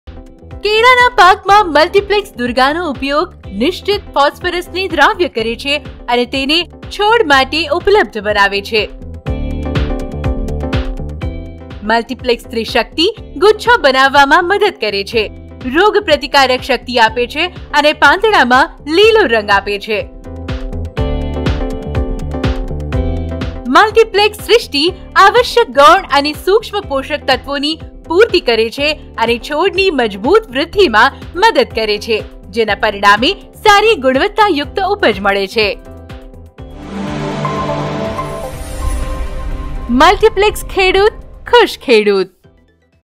Multiplex Products for Banana Farmers I Multiplex Durga I Multiplex Trishakti I Multiplex Srushti
કેળાના પાકમાં મલ્ટીપ્લેક્સ દુર્ગાનો ઉપયોગ નિશ્ચિત ફોસ્ફરસને દ્રાવ્ય કરે છે અને તેને છોડ માટે ઉપલબ્ધ બનાવે છે. મલ્ટીપ્લેક્સ ત્રિશક્તિ ગુચ્છો બનાવવામાં મદદ કરે છે, રોગ પ્રતિકારક શક્તિ આપે છે અને પાંદડામાં લીલો રંગ આપે છે. મલ્ટિપ્લેક્સ સૃષ્ટિ આવશ્યક ગૌણ અને સૂક્ષ્મ પોષકતત્વોની પૂર્તિ કરે છે અને છોડની મજબૂત વૃદ્ધિમાં મદદ કરે છે જેના પરિણામે સારી ગુણવત્તાયુક્ત ઉપજ મળે છે.

Application of Multiplex Durga in banana crop solubilizes fixed phosphorus and makes it available to plants. Multiplex Trishakti helps in bunch formation, induces disease resistance and imparts green colour in leaves. Multiplex Srushti supplements essential secondary and micronutrients and helps in robust growth of the plants resulting in better quality yield.